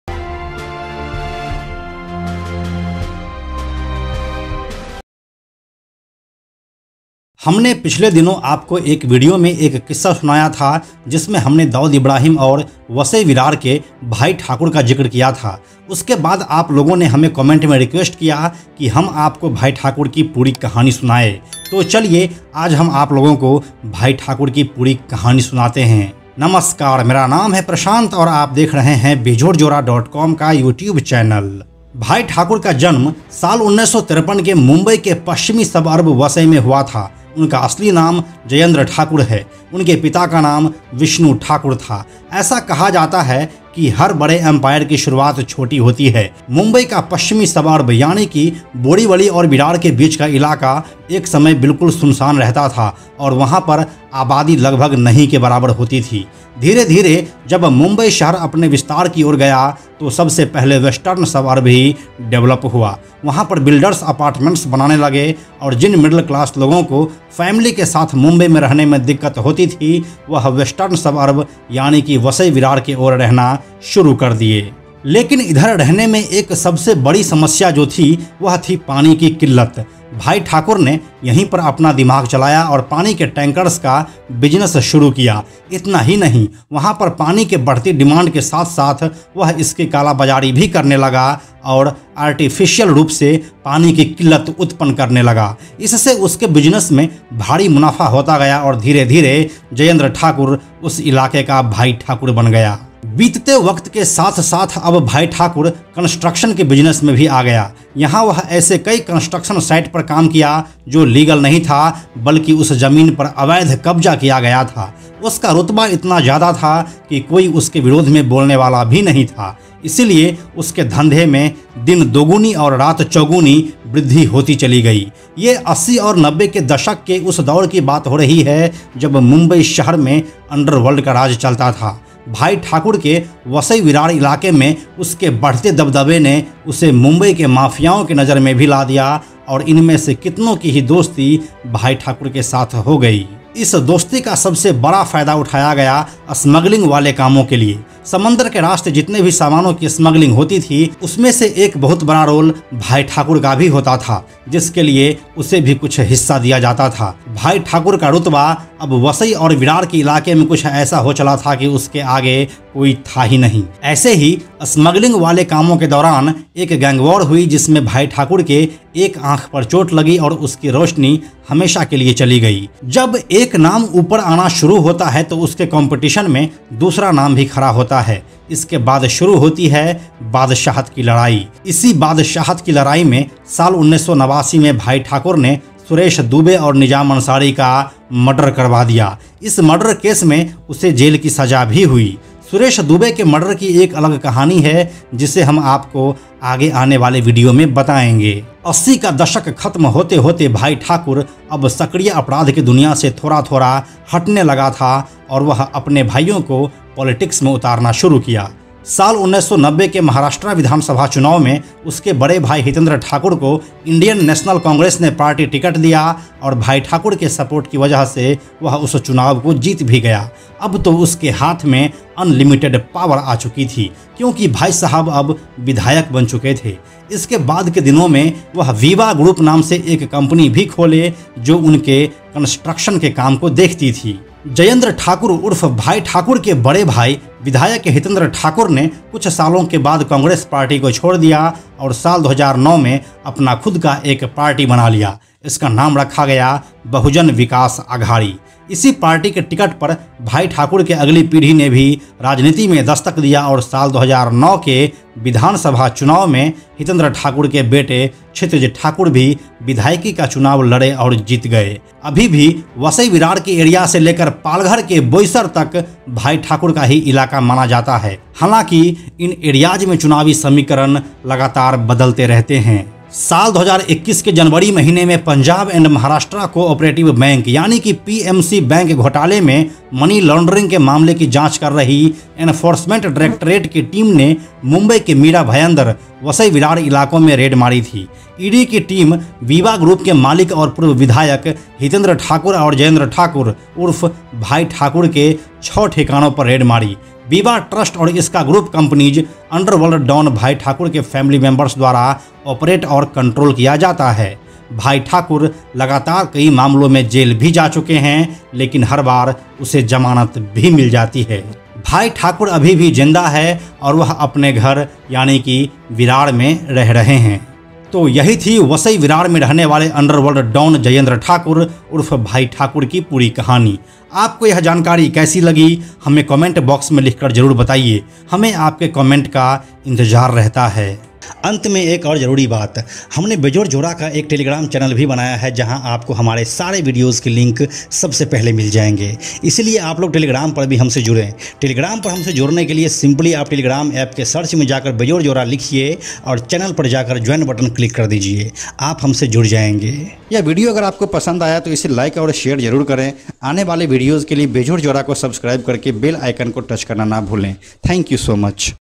Vasai-Virar के Bhai Thakur की पूरी कहानी | Hitendra Thakur | Mumbai Underworld | Bejod Joda
About the Video: बीतते वक्त के साथ-साथ अब भाई ठाकुर कंस्ट्रक्शन के बिजनेस में भी आ गया। यहाँ वह कई ऐसे कंस्ट्रक्शन साइट पर काम किया जो लीगल नहीं था बल्कि उस जमीन पर अवैध कब्जा किया गया था। उसका रुतबा इतना ज्यादा था कि कोई उसके विरोध में बोलने वाला भी नहीं था।

हमारे कुछ और दिलचस्प और जानकारीपूर्ण वीडियो को बिलकुल भी मिस ना करें:

Video credit:

Concept: Barun Kumar
Research: Prashant Kumar
Executed By: Barun Kumar
Editor & Voice Over: Prashant Kumar
Produced By: Bejod Joda